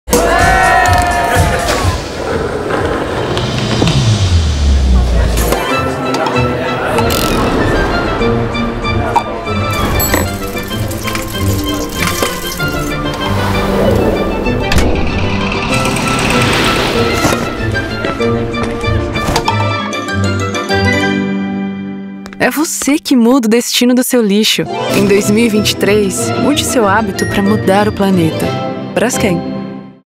Em 2023, mude seu hábito para mudar o planeta.​
A Braskem convida a única pessoa capaz de​
mudar o destino do seu lixo: você. Vamos juntos?